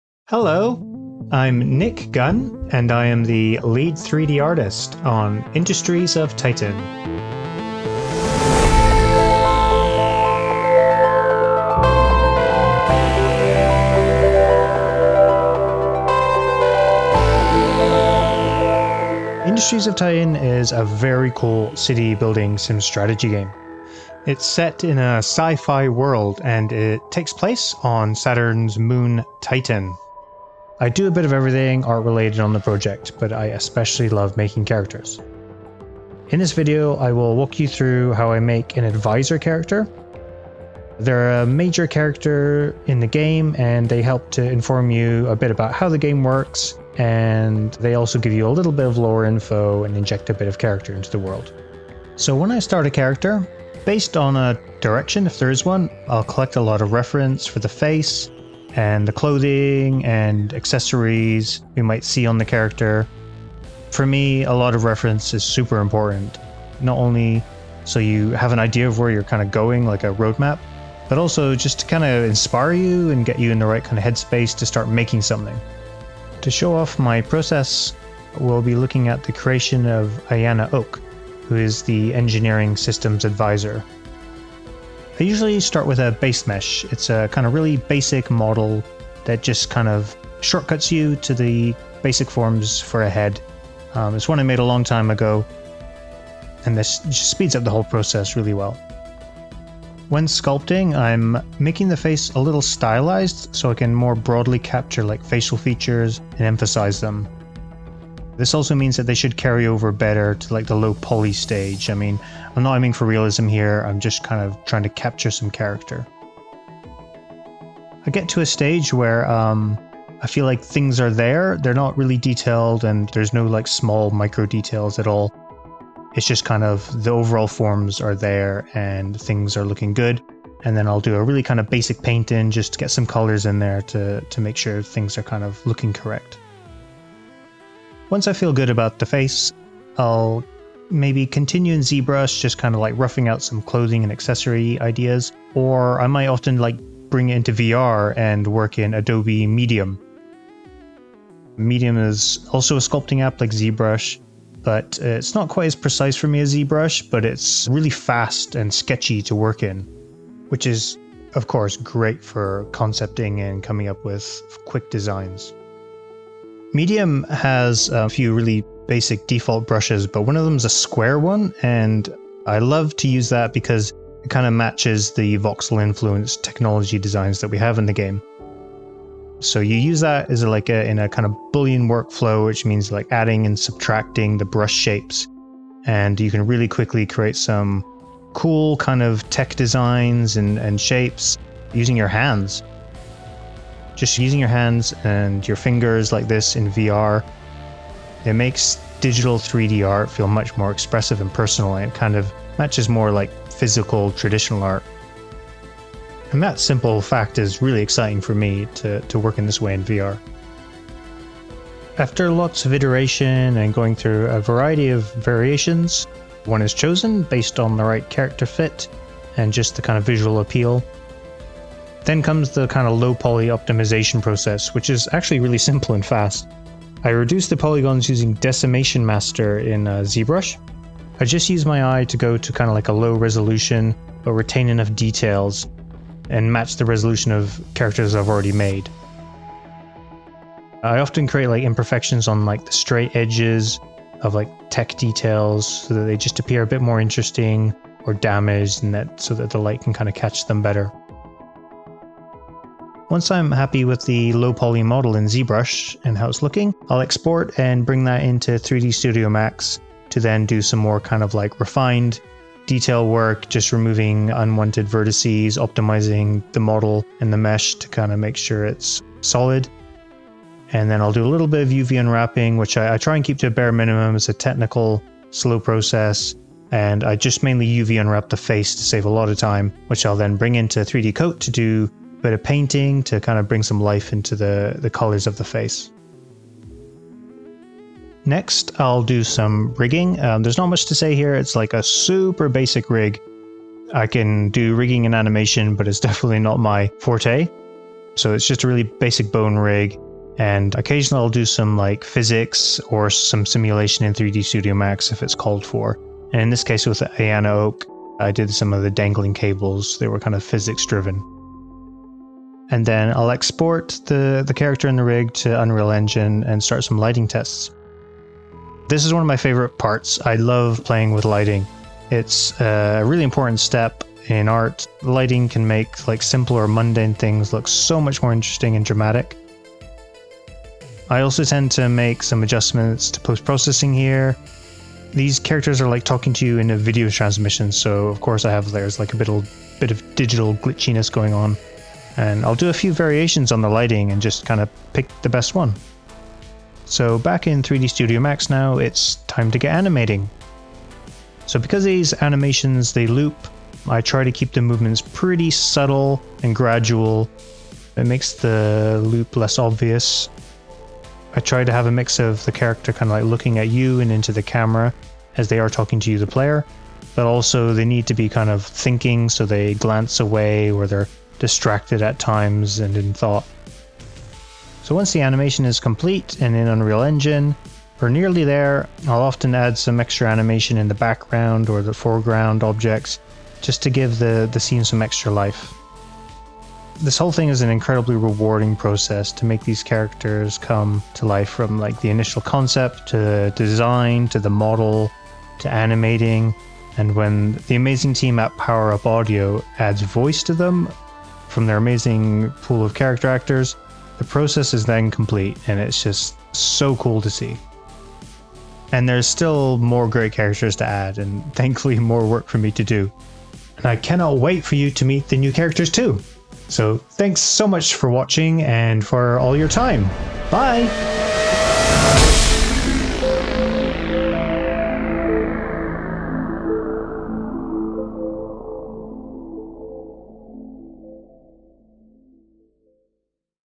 Behind the Scenes with Nick Gunn: 3D Character Art Process in Industries of Titan
Nick Gunn, 3D Artist at Brace Yoruself Games, peels back the curtain to show you what goes in to creating your Titan advisors.